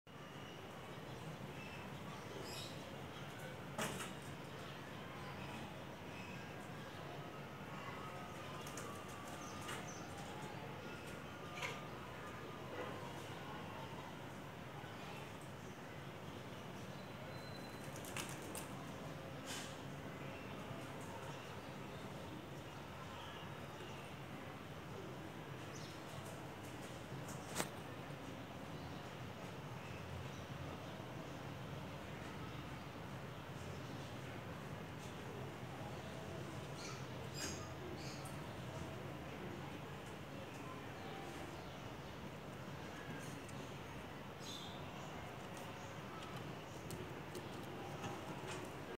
Spying on my cats when they're intruding birds' nest
The birds were out so no worries -